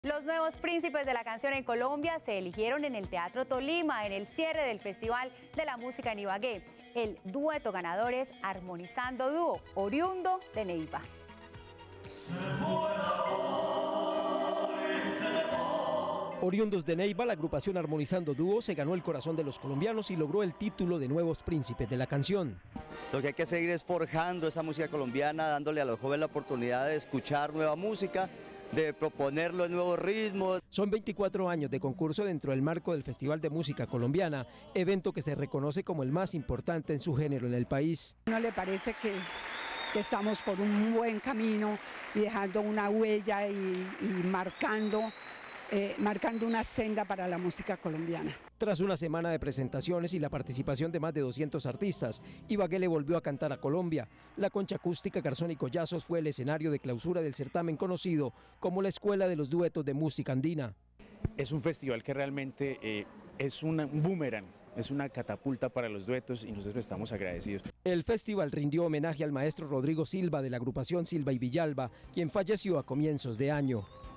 RED+ | Dueto “Armonizando dúo” nuevos príncipes de la canción en Colombia
Los nuevos príncipes de la canción en Colombia se eligieron en el teatro Tolima, en el cierre del festival de la música en Ibagué...el dueto ganador es "armonizando duo" oriundo de Neiva…